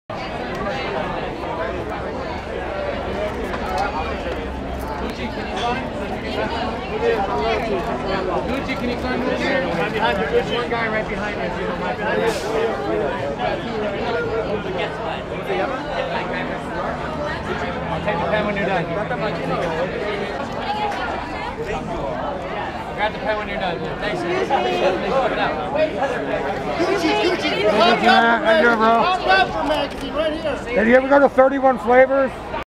Gucci Mane and Keyshia Dior greet fans at the Spring Brea...
Gucci Mane and Keyshia Dior greet fans at the Spring Breakers After Party at the Emerson Theatre in Hollywood, 03/14/13